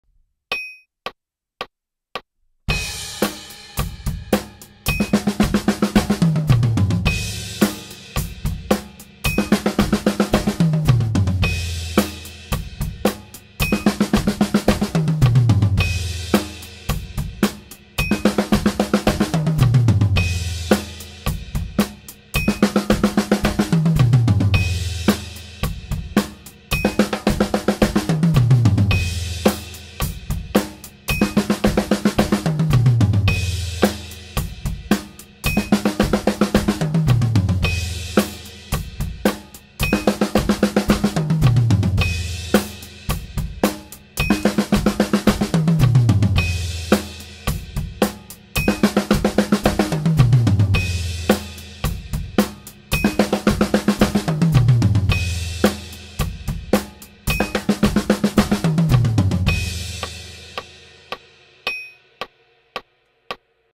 一つ打ちのタム回し（♩＝110）/ 2023.07.09
腕の重さや身体の重さが
しっかりと音に乗るようにしたい